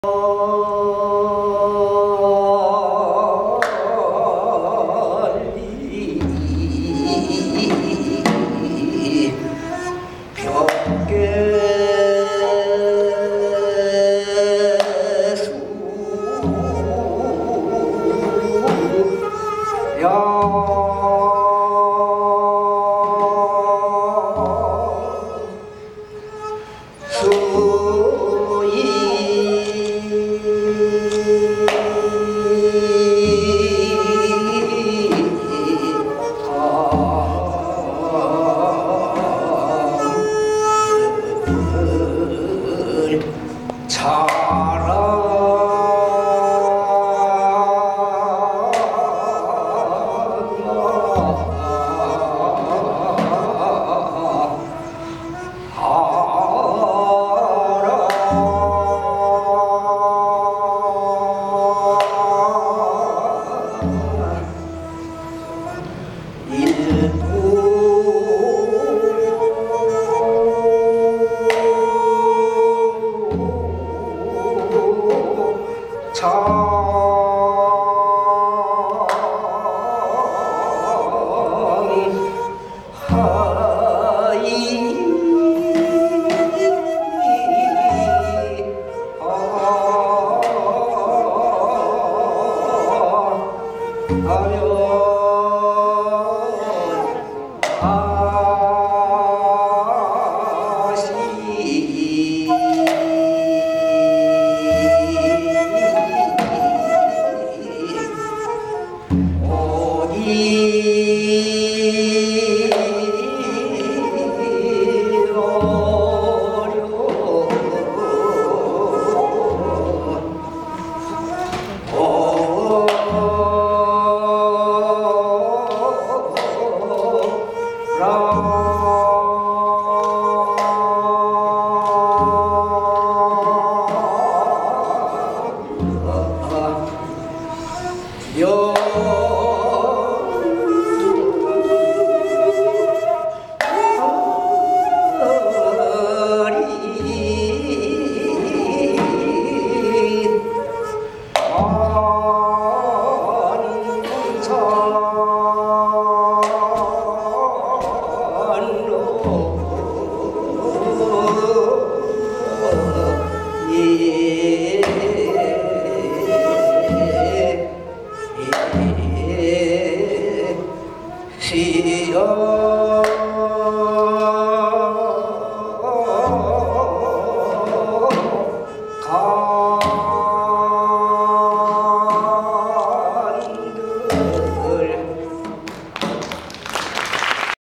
작전및근무기강확립400일달성감사페스티벌 평시조
무궁화꽃예술단은 각분야에서.활동하시는 분들로 자진구성된.단체입니다..2017.3.3.금.서산예비군훈련소 오후3.30분공연작전및근무기강확립400일달성감사페스티벌